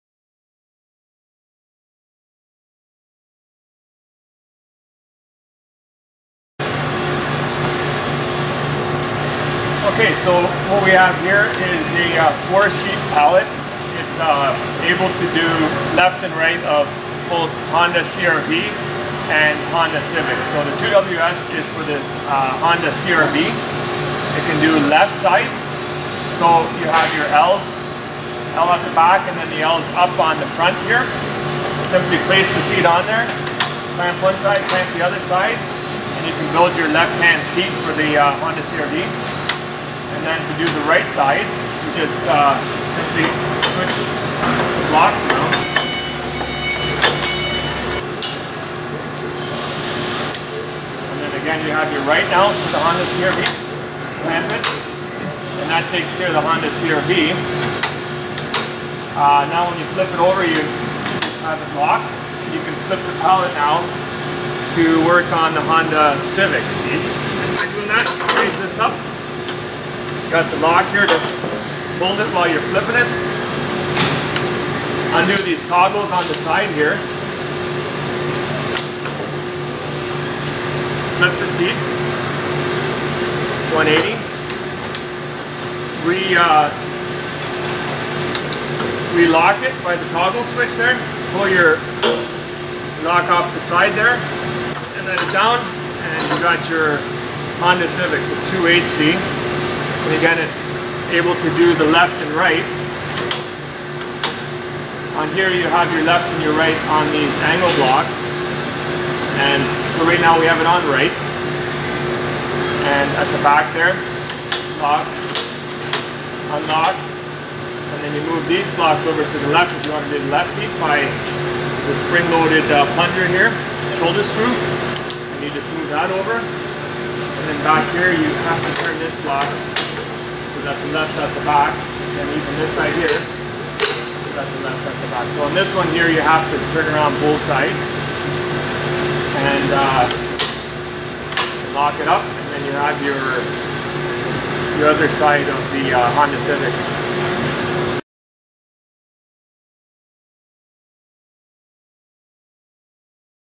CMP: Multi Seat Build Pallet Tooling
CMP builds pallets and tooling for the seating industry conveyor based assembly lines.  This unique pallet tooling design allows manufacturers to assemble 4 different seat types on the same pallet.  All changeover steps are tool-less and quick change.

CMP Automation Inc.